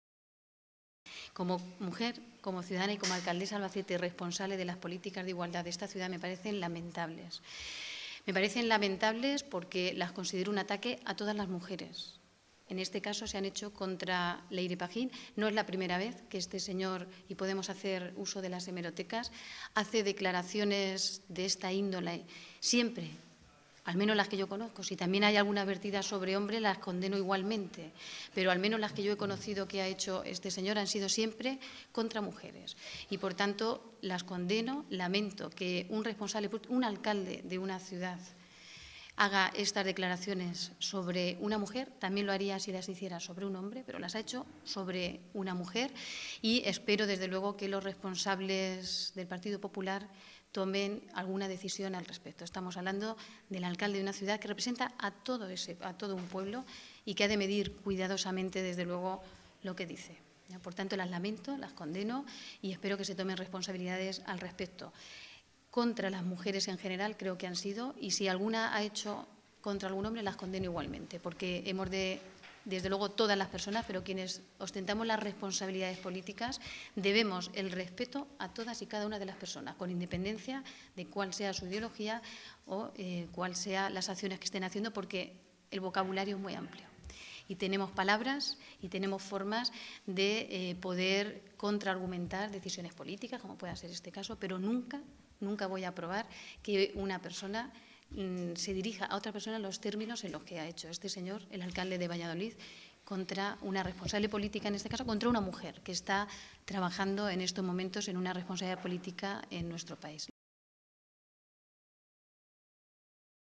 TOTAL ALCALDE DE VALLADOLID
Esta es la opinión del Alcalde de Valladolid sobre la nueva Ministra de Sanidad, Leyre Pajín.